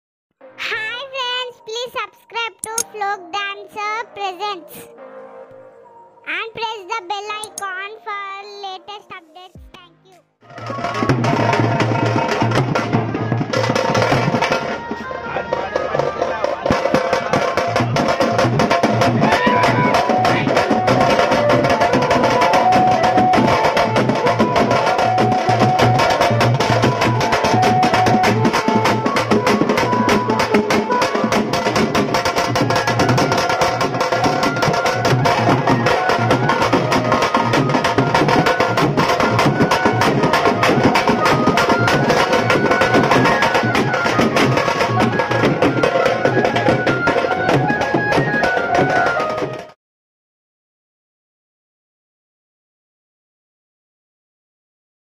TeenMaar Dance Steps On Chatal Band | Abhilash Pad Band | Lothkunta Congo Tinku Durga Visarjan 2020
Any Videos To be Uploaded In Our Youtube Channel

© Copyright - Folk Dancer Presents 2020